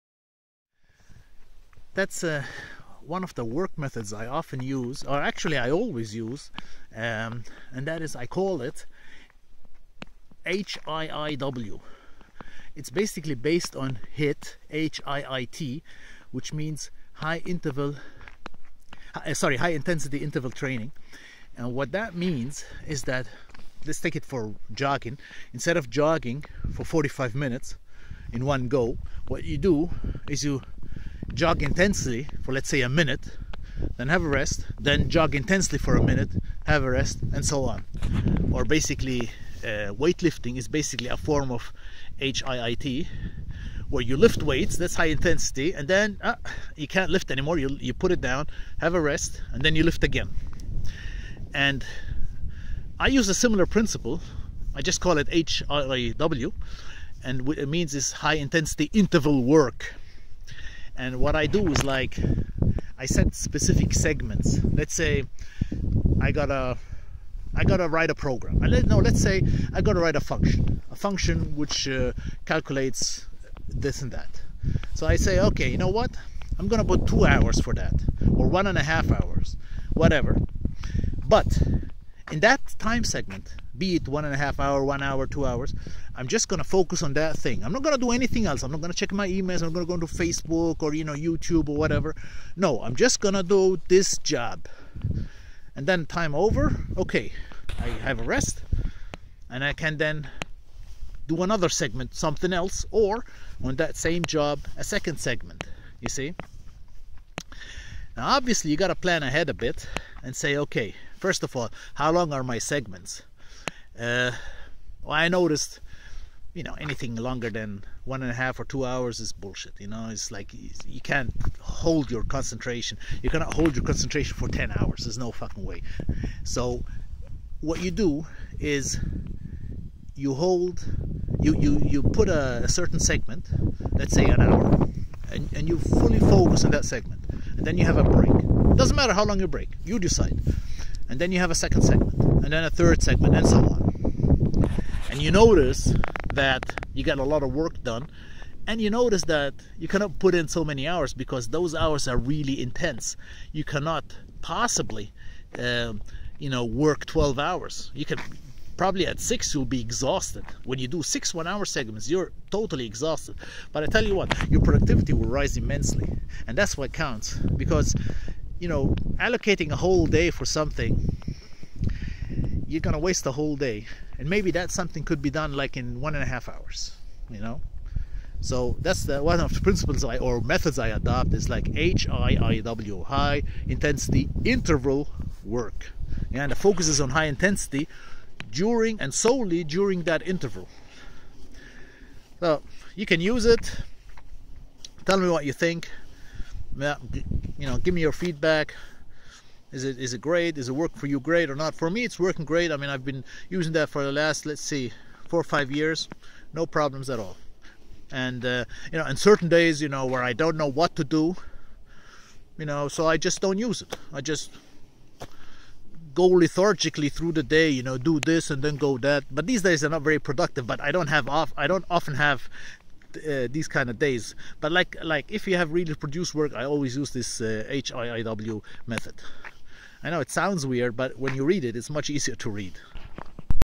HIW (High Intensity Interval Work) Method based on HIIT (High Intensity Interval Training)
Divide you day into segments.

Optimally each segment should be between 45 to 90 minutes.
Longer segments come with in a risk of losing focus.

Each segment consists of very intense deep work without any distractions

Put in breaks between each segment